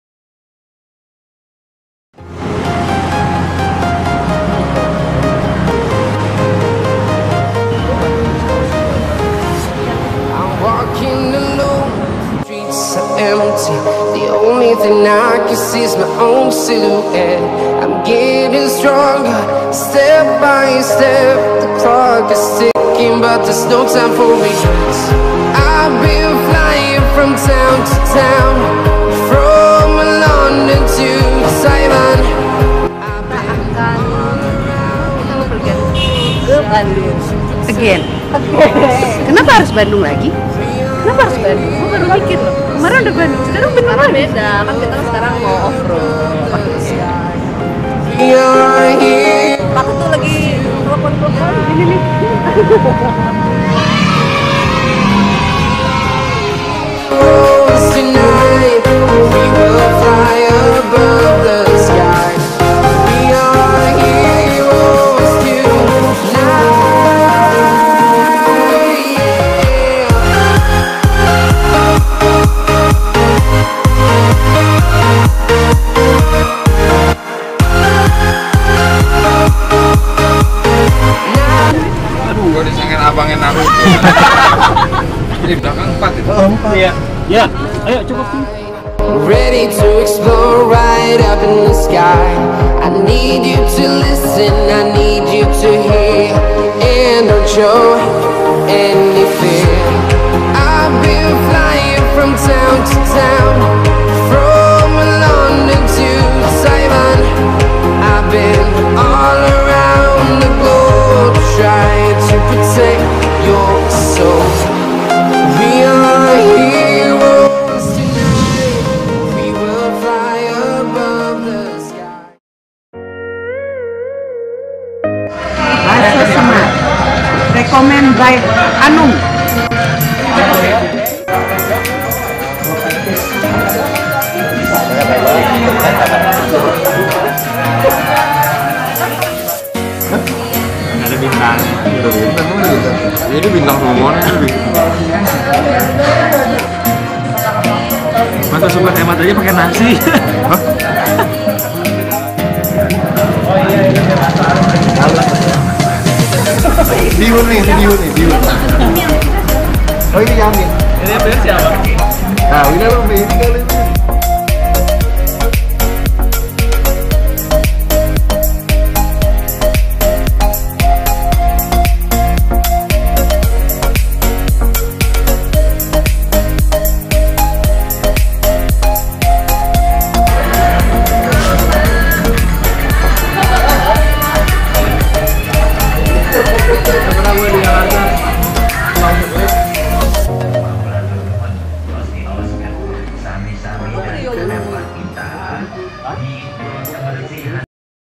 In Action Squad Adventure part - 2 Keberangkatan
hey we're come back...

Setelah 1th yang lalu kita berpetualang di bukit moko, sekarang kita coba berpetualang yang agak 'menantang" Offroad di cikole (bandung), asik,dan menyenangkan..

tapi sayangnya kita gak lengkap, kurang 2 orang lagi...

kita berharap tim In Action dapat berkumpul Komplit..

mudah-mudahan next trip..kita semua kumpulllllll

video ini kita bagi per Part 1 s/d 7, irit kuota juga hehehe

jakarta12-14 oktober 2018